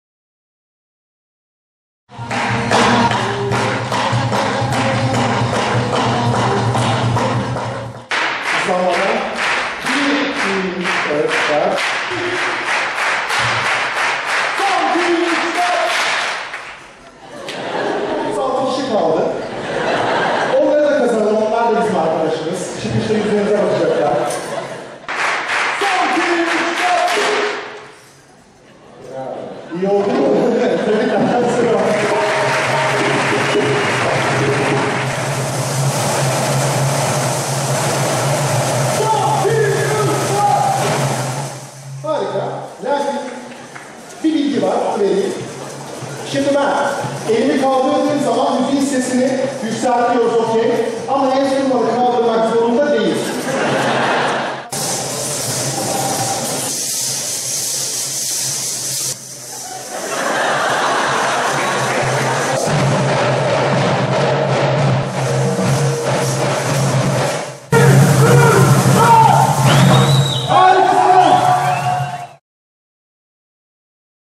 DeFacto - Ritm Motivasyon Atölyesi
M3 Works ekibi olarak DeFacto ekibi ile gerçekleştirdiğimiz Ritm Motivasyon Atölyesi etkinliği keyifli anlara sahne oldu. Toplam 1000 katılımcıya sürpriz yapılarak gerçekleştirilen etkinlikte hiçbir şeyden haberi olmayan katılımcıları etkinlik alanına geldiğinde ritm aletlerini görünce çok şaşırdılar. Keyifli geçen etkinlikte eğlenirken takım olmanın, birlikte hareket etmenin ne kadar önemli olduğunu bir kez daha hatırlamış oldular.